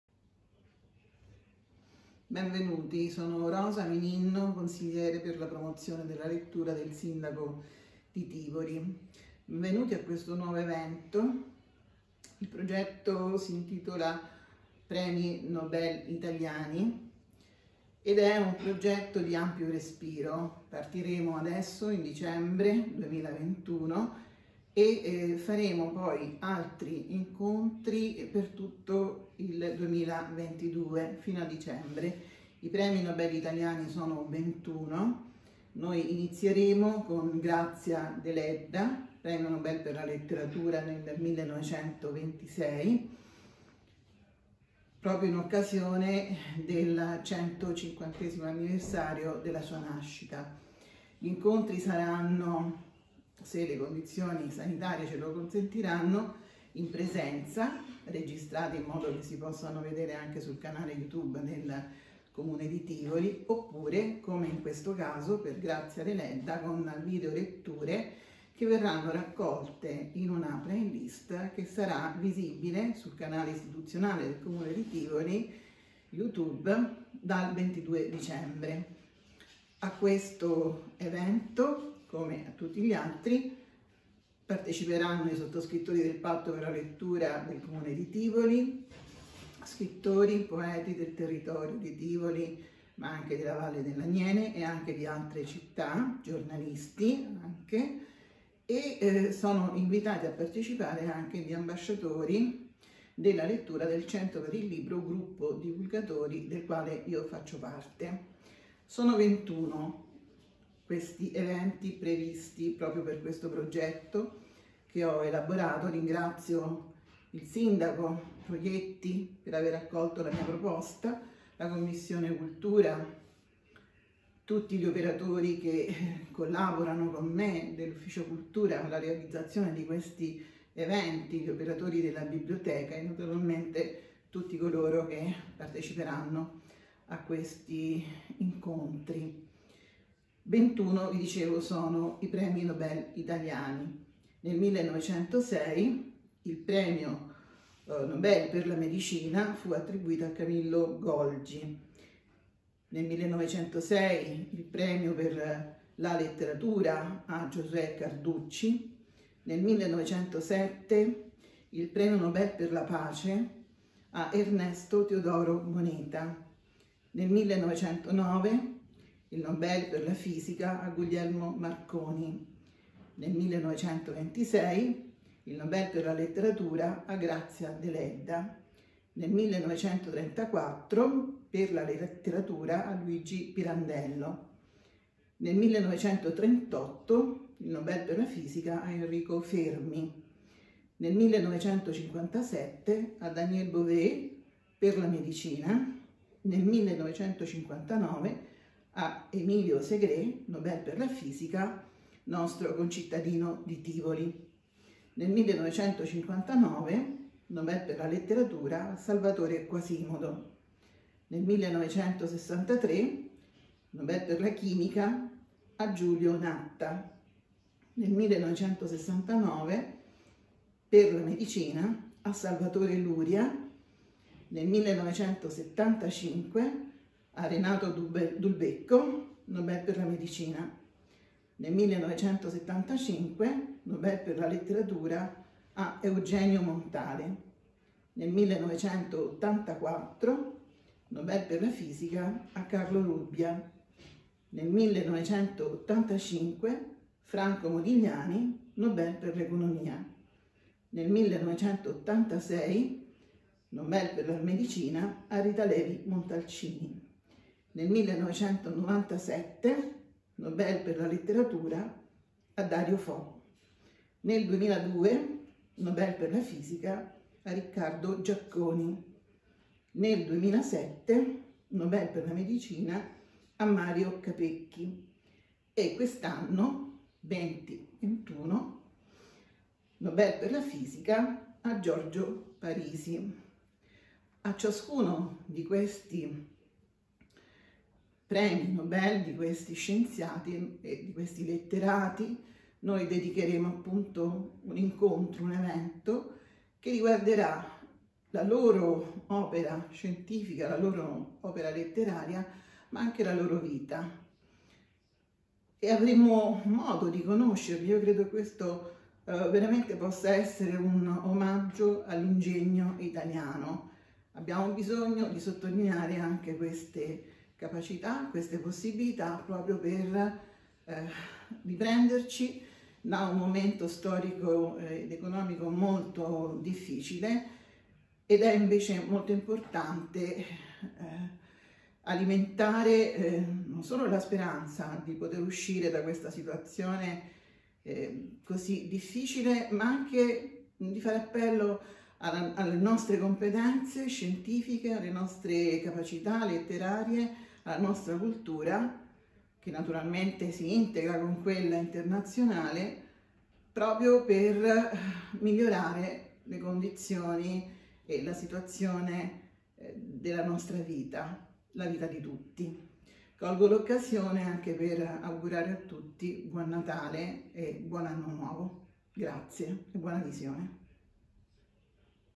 Premi Nobel Italiani 2021-22 Rosa Mininno Consigliere del Sindaco per la promozione della lettura.
Presentazione del progetto "Premi Nobel Italiani" da Dicembre 2021 a Dicembre 2022
primo evento 22 Dicembre videoletture dedicate alla vita e alle opere di Grazia Deledda nel 150° anniversario della sua nascita.